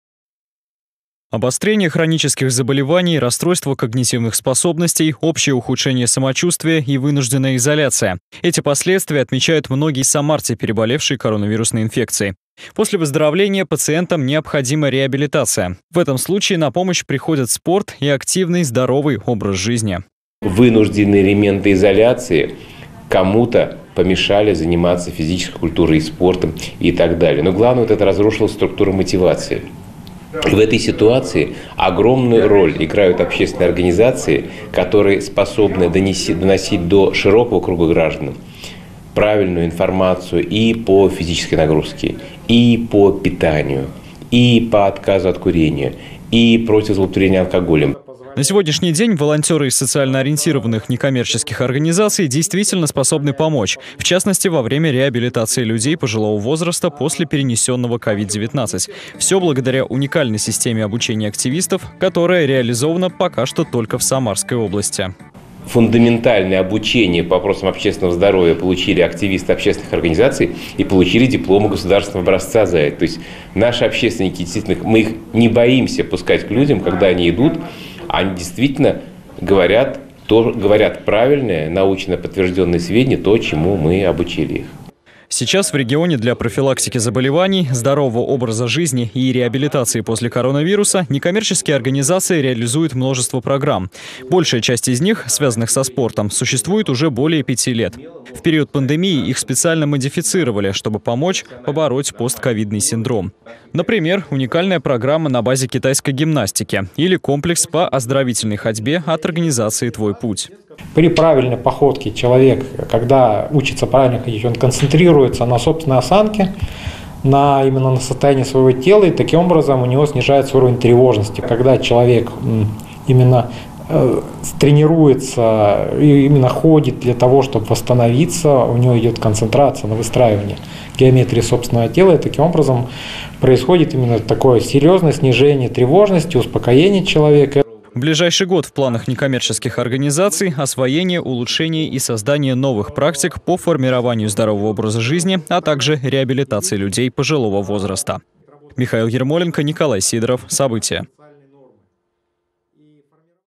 В Самарской области будут развивать НКО, которые популяризируют здоровый образ жизни
Профилактика заболеваний, здоровый образ жизни и реабилитация после перенесенной коронавирусной инфекции. Члены общественной палаты Самарской области обсудили роль и перспективы развития некоммерческих организаций региона по трем этим приоритетным направлениям. О намеченных целях в работе и итогах реализации проектов в период пандемии расскажет Михаил Ермоленко.